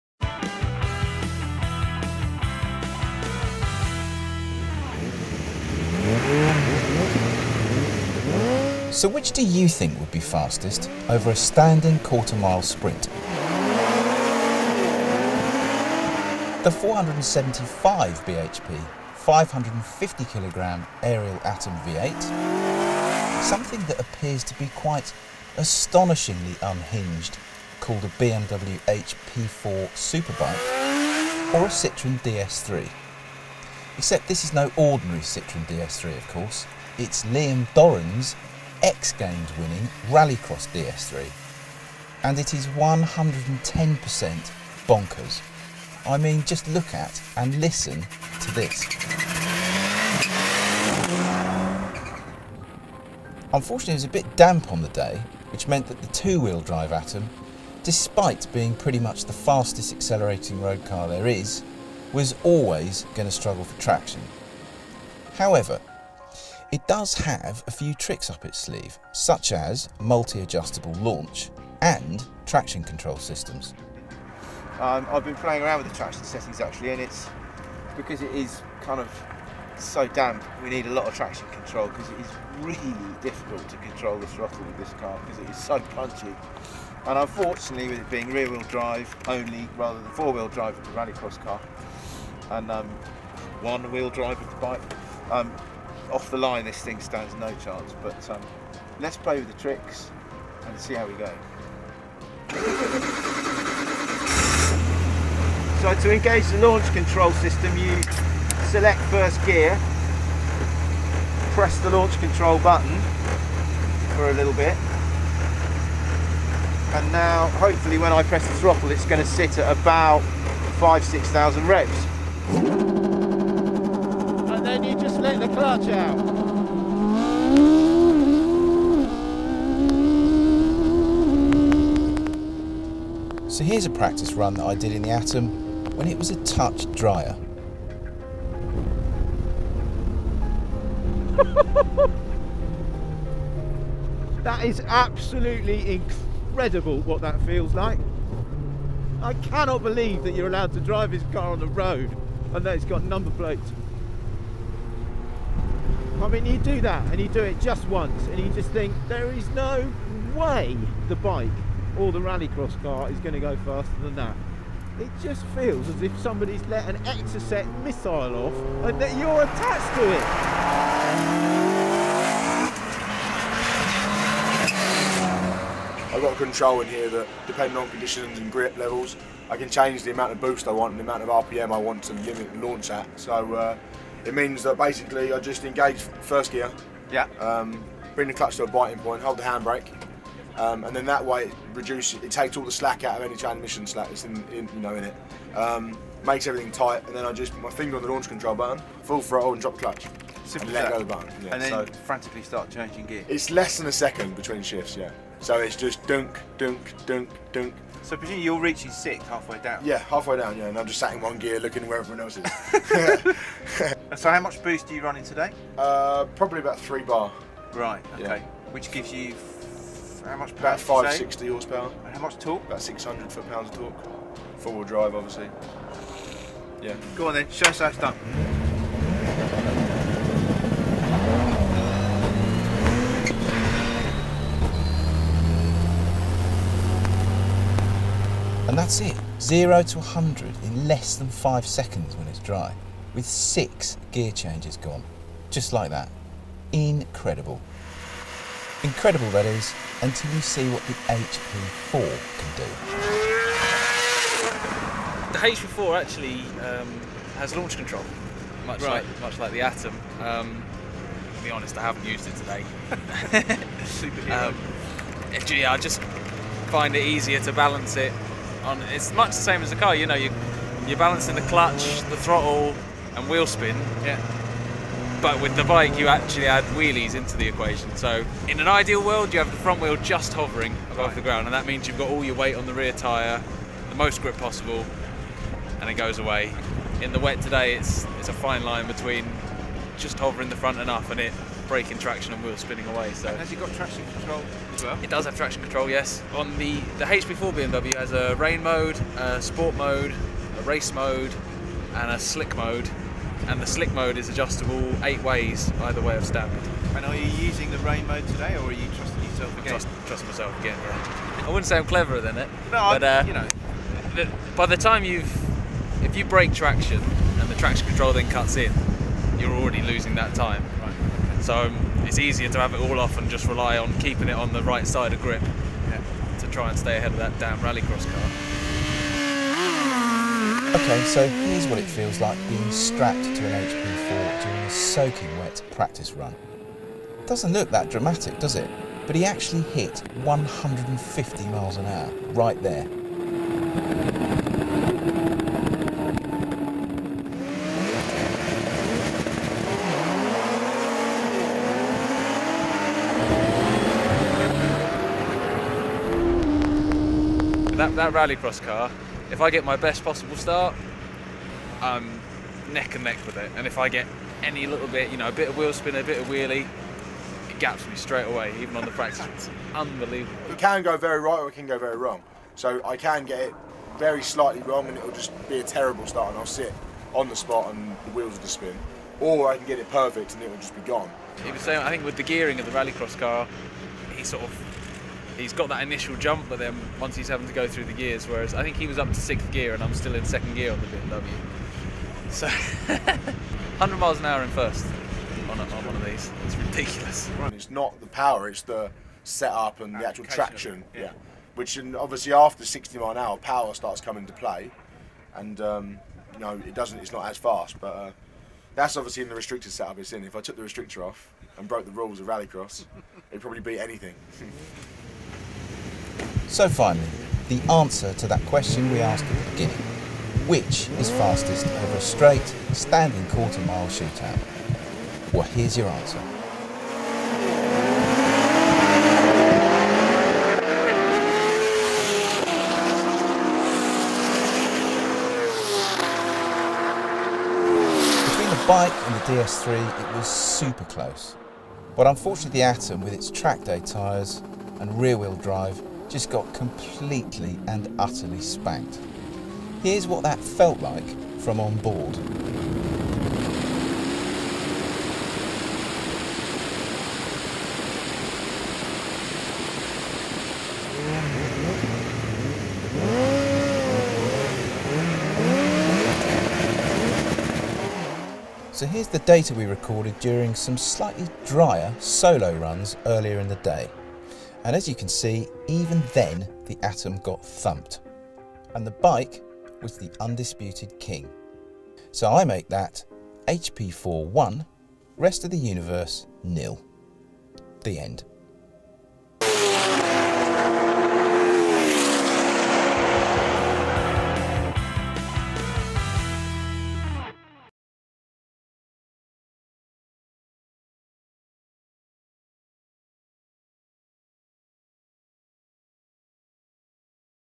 The sports cars Ariel Atom V8 vs 600bhp rallycross Citroen DS3 vs BMW HP4 superbike drag race
Which is faster? An Ariel Atom V8 track day toy, BMW HP4 superbike or a rallycross Citroen DS3 with 600bhp?

Here we have three completely different ways of going extremely quickly in a straight line. Time for a drag race then. So what are the contenders? There's the one-wheel drive BMW HP4 superbike, which arguably requires the most skill to extract speed from but has a ludicrous 757bhp/tonne power-to-weight ratio. Then there's the two-wheel drive Ariel Atom V8 with 748bhp/tonne and the Citroen DS3 Rallycross car with only 577bhp/tonne but all-important four-wheel drive. Oh, and did we mention it's wet?

What would you like to see go head-to-head in our next drag race?